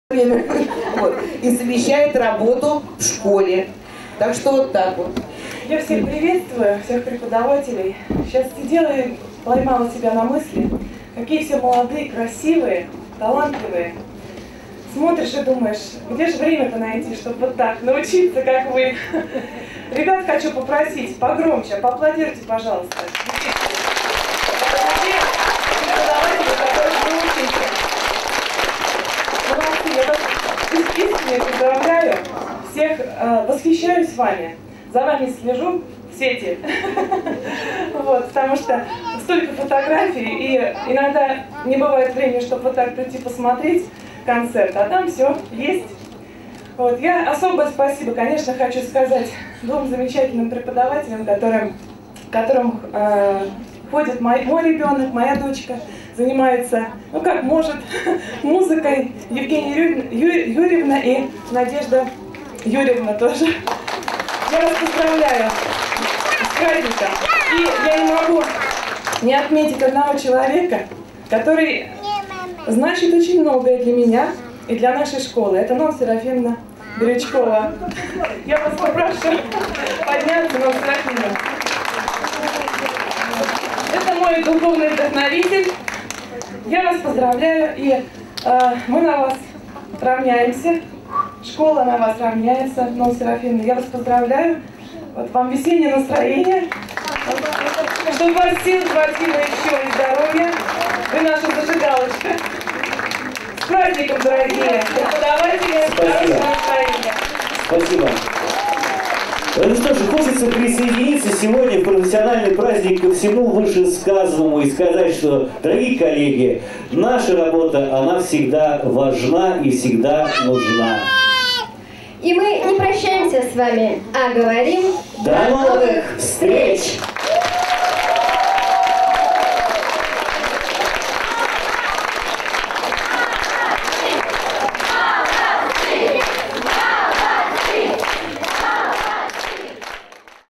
Часть 5. Концерт, посвященный Дню работника культуры. Центр культуры и досуга, д. Путилково
Пятая и последняя часть концерта, посвященного Дню работника культуры, который состоялся в Центре культуры и досуга сельского поселения Отрадненское (Красногорский район Московской области) 24 марта 2017 года.

Выступление директора Центра Елены Дергаусовой
Выступление педагогов Ульяновской СШ №20